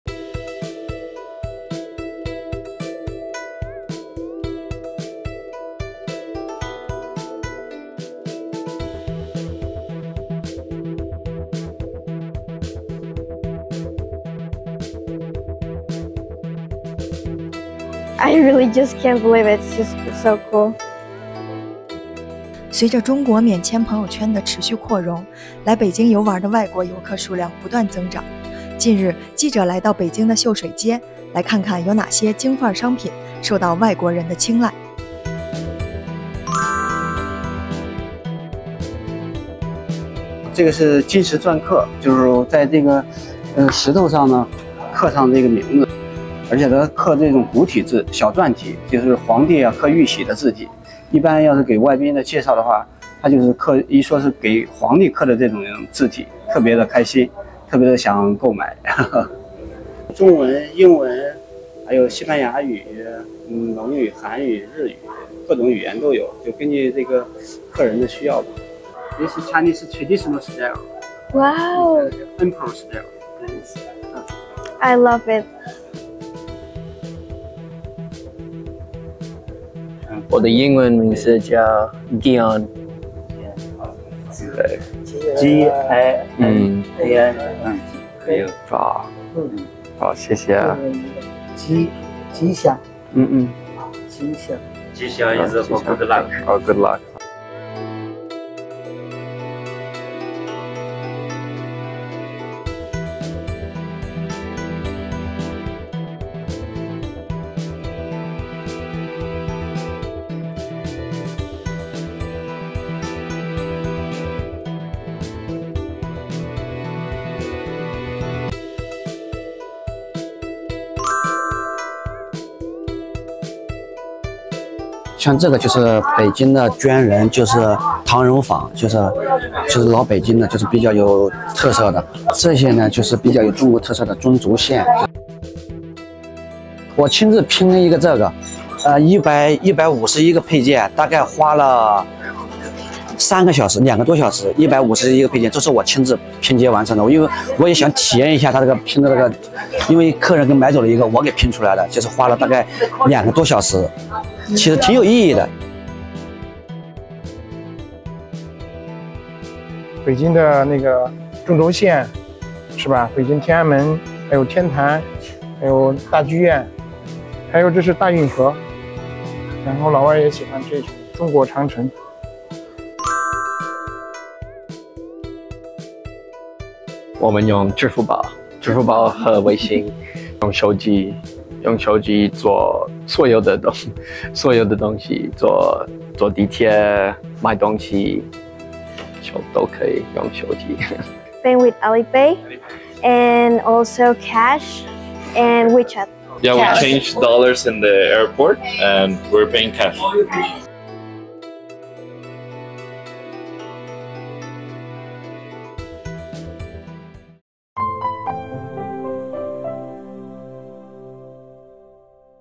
来看看外国人青睐哪些“京范儿”商品？
来自哥伦比亚的小游客Mariana，得到了一个篆刻有她中文名字的印章，她直呼“太酷了”。会说中文的澳大利亚小伙Gian，从老板那里得知，自己中文名“吉安”的“吉”寓意着“好运”。

随着中国免签“朋友圈”的持续扩容，来北京游玩儿的外国游客数量不断增长。近日，记者来到北京的秀水街，来看看有哪些“京范儿”商品受到外国人的青睐？（熊然）